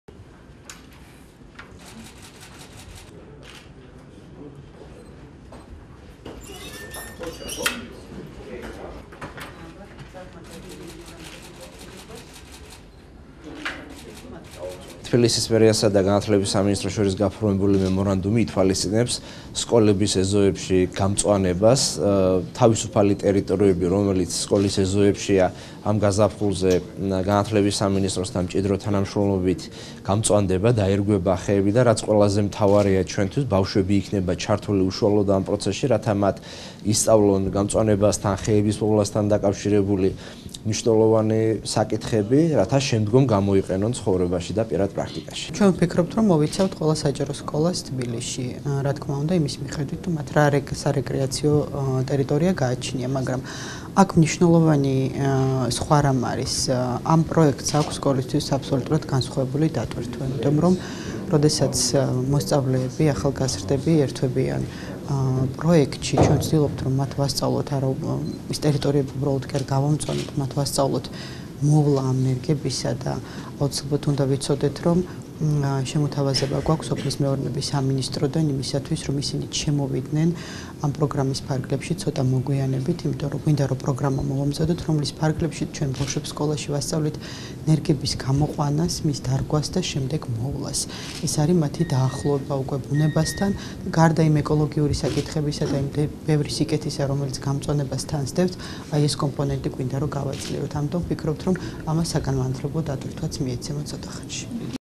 ქალაქ თბილისის მერიასთან ურთიერთთანამშრომლობის მემორანდუმი გაფორმდა
საქართველოს განათლებისა და მეცნიერების სამინისტროსა და ქალაქ თბილისის მერიას შორის ურთიერთთანამშრომლობის მემორანდუმი გაფორმდა.

დედაქალაქის მერიაში მემორანდუმს ხელი მოაწერეს საქართველოს განათლებისა და მეცნირების მინისტრმა თამარ სანიკიძემ და თბილისის მერმა დავით ნარმანიამ.

მემორანდუმის მიზანია, ქალაქ თბილისის მუნიციპალიტეტში არსებულ ზოგადსაგანმანათლებლო დაწესებულებების ტერიტორიაზე, ურთიერთთანამშრომლობის გზით, გარემოს დაცვის, ეკოლოგიური უსაფრთხოების, გარემოსდაცვითი საკითხების პოპულარიზაციისათვის გამწვანებითი ღონისძიებების ჩატარება, ზოგადსაგანმანათლებლო დაწესებულებებში გარემოსდაცვითი განათლების ხელშეწყობა და მოსწავლეთა შორის ეკოლოგიური საკითხების პოპულარიზაცია.